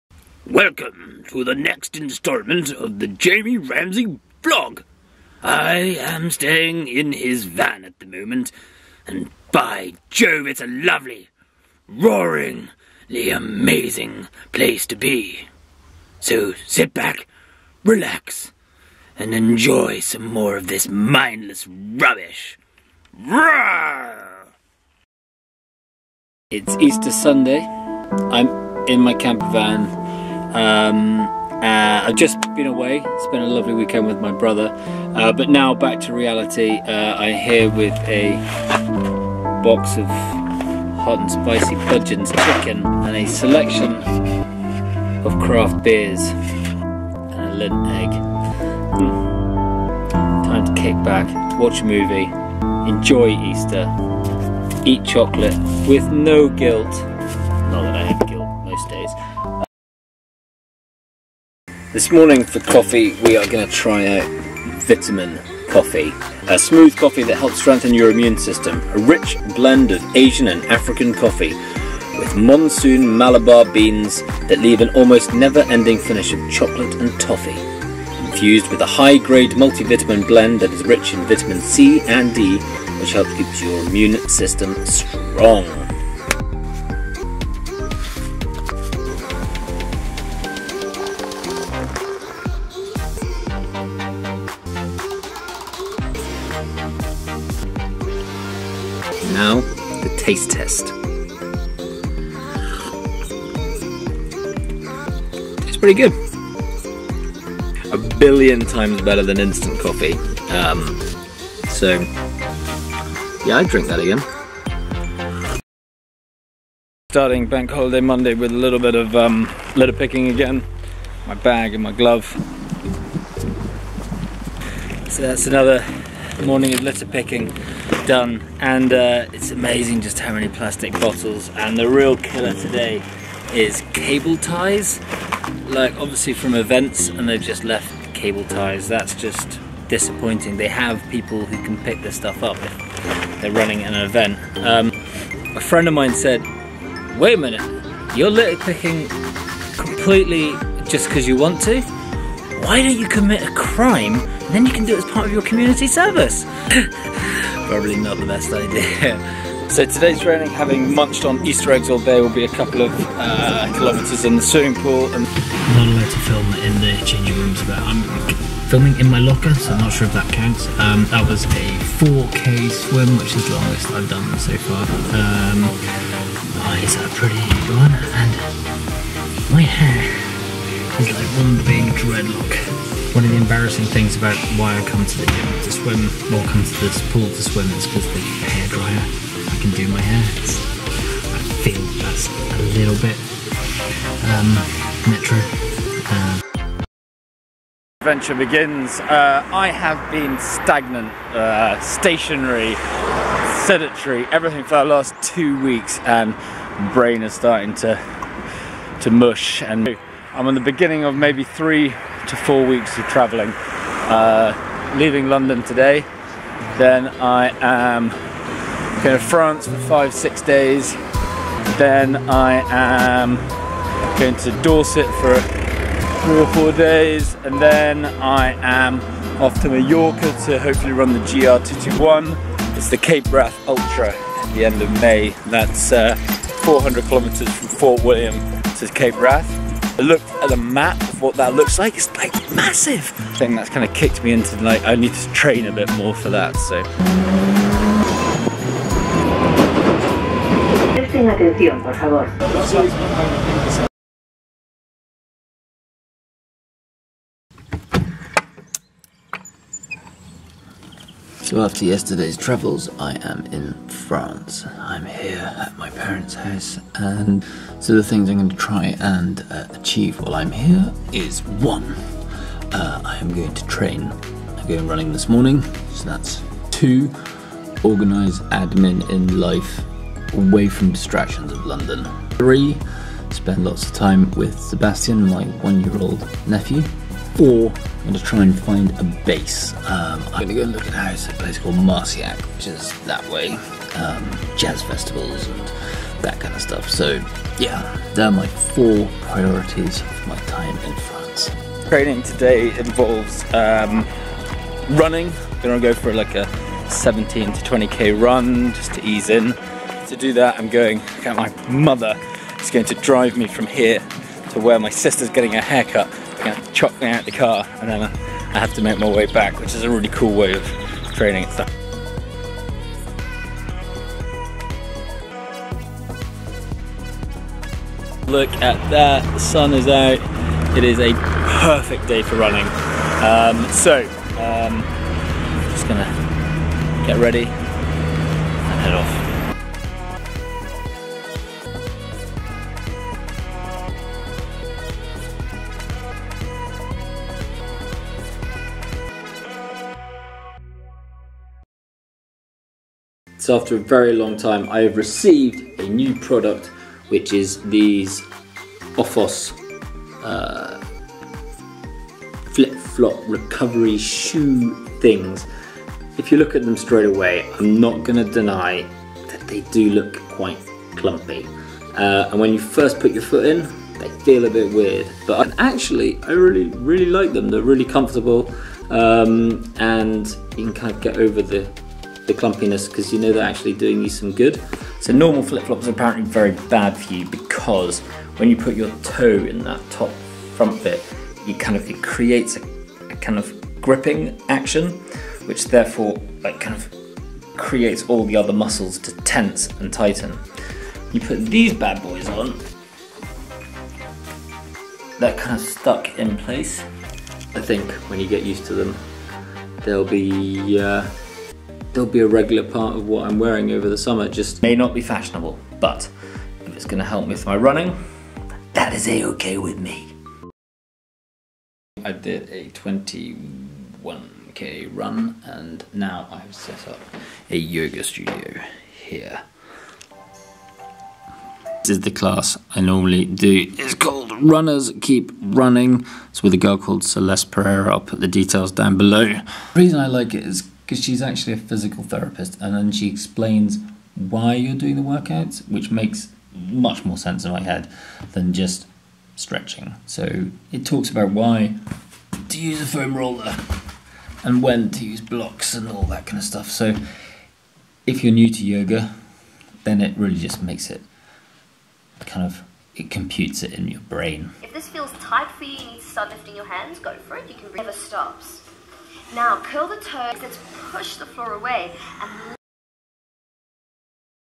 VLOG 0704: Dinosaurs, Easter Eggs, Travel, Rubbish, Oofos, Vitamin coffee and France
This is a bit of a collection of videos from the week. It starts with a T-Rex intro and Easter Sunday in a campervan and ends in France. There is some rubbish talk and training in between as well as a taste test of Vitamin Coffee and Oofos flip flops.